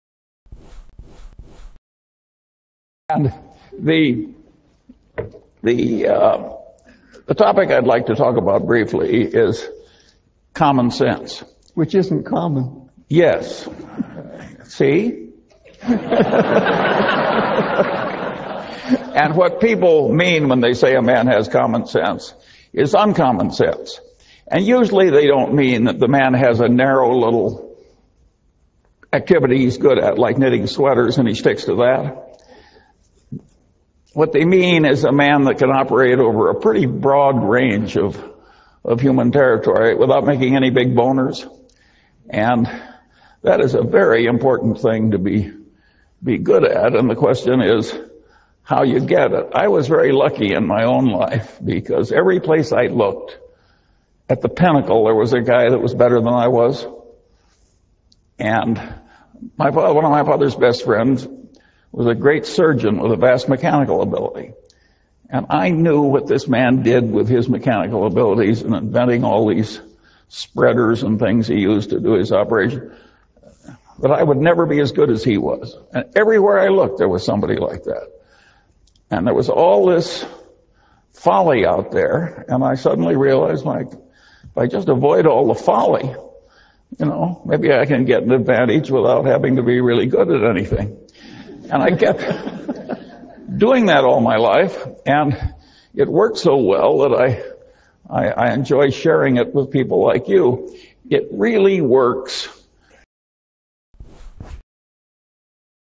Charlie Munger on Common Sense
Charlie begins his interview with Tom Tombrello about common sense which isn't all that common.

Taken from his hour with Caltech students in 2008.

📚The Little Book of Common Sense Investing - John Bogle

(NL)

Keep thinking,

The Free Bull